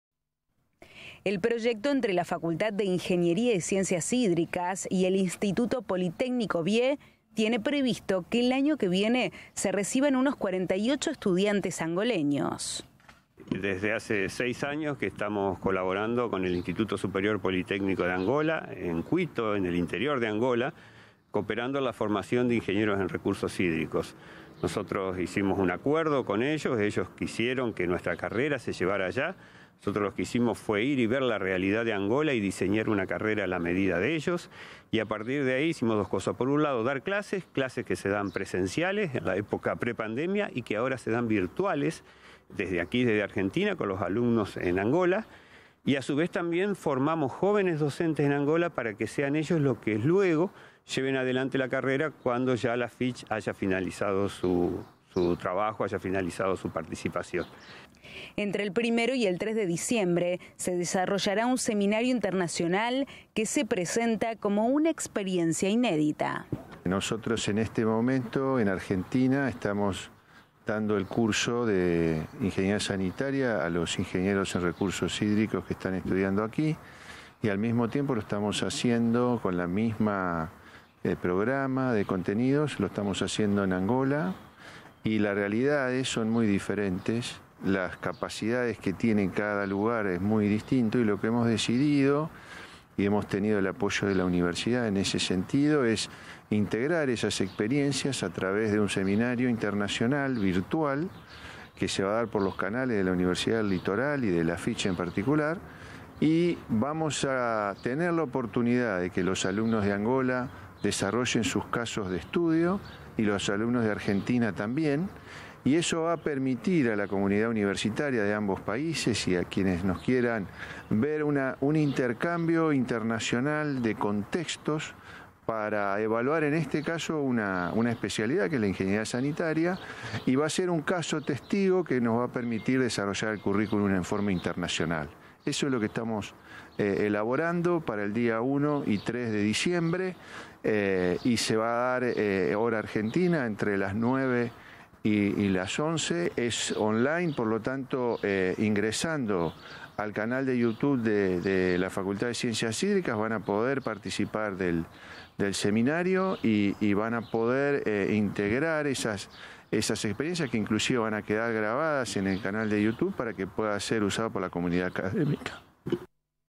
La Facultad de Ingeniería y Ciencias Hídricas continúa formando a profesionales de Angola
Se trata de un convenio de la Casa de Estudios, de la Universidad Nacional del Litoral, con estudiantes de este país africano.